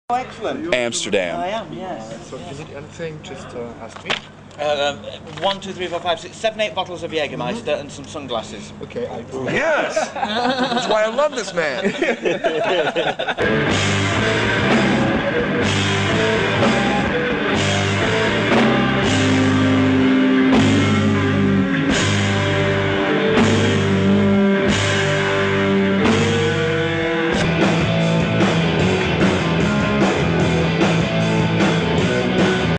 Bare Jr in Amsterdam
Bare Jr and the Immortal Records tour crew backstage in Amsterdam 1999